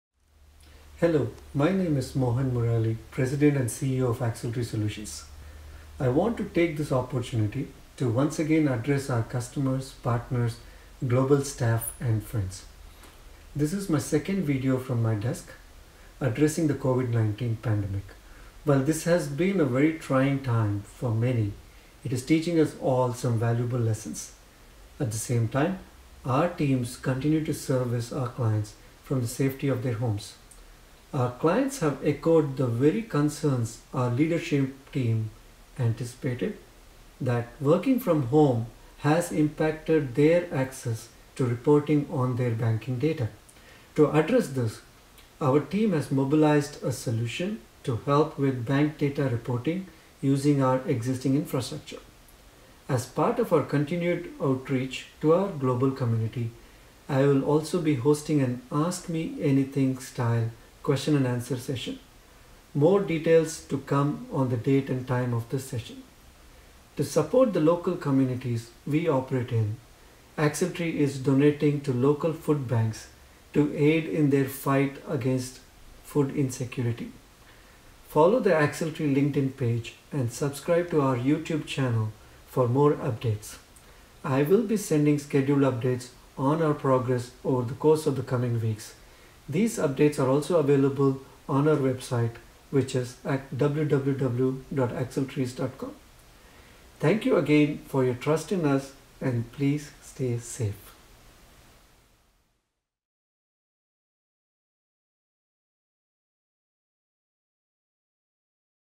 President's Message - COVID 19 - Update 2
April 1 COVID-19 Video update from Axletree Solutions CEO Mohan Murali. Find your local bank by using this link.